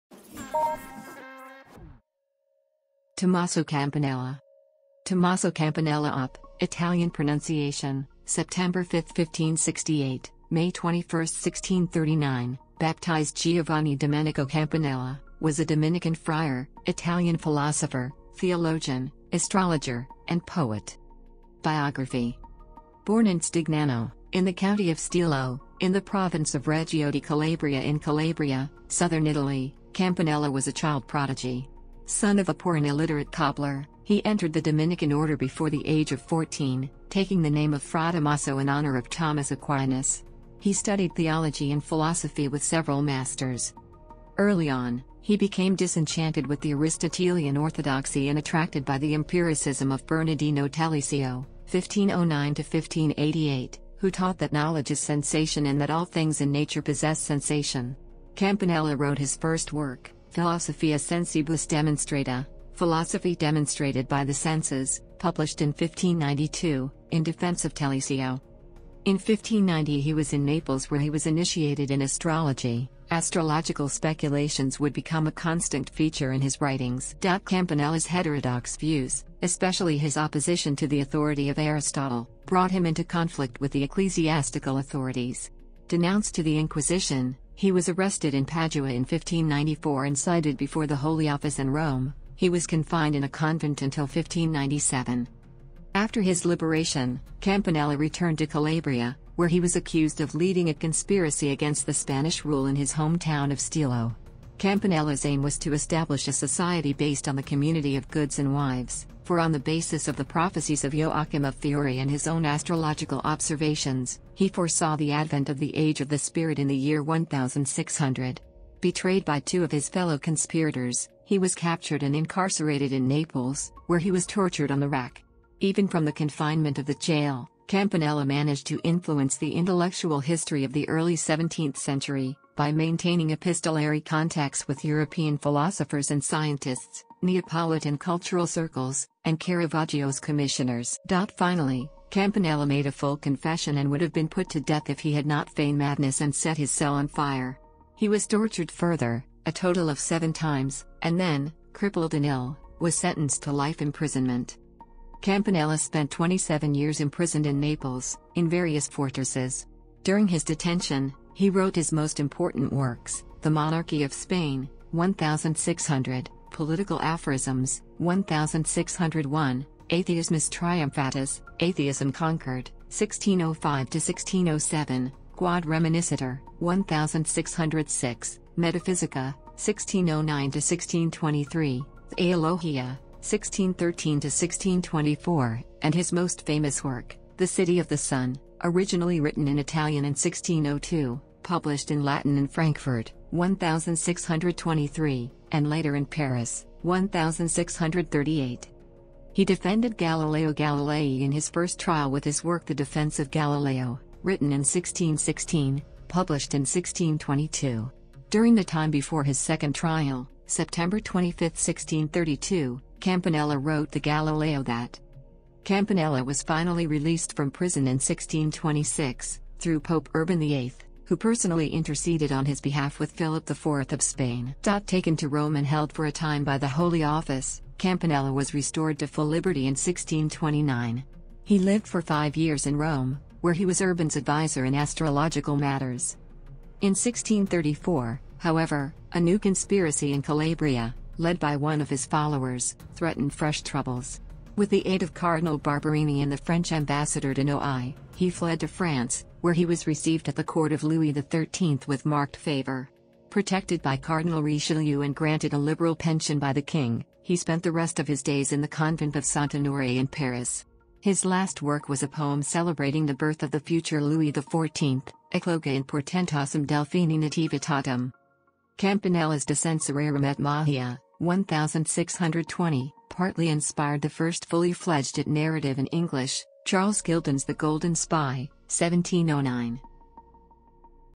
Tommaso Campanella 👩‍🏫📜 Everything Philosophers 🧠👨🏿‍🏫
Books on Philosphy
Philosophy Books for Kids
Explore the world with this Digital Microscope
Explore the sky with this Telescope

Tommaso Campanella OP (Italian pronunciation: ; 5 September 1568 – 21 May 1639), baptized Giovanni Domenico Campanella, was a Dominican friar, Italian philosopher, theologian, astrologer, and poet.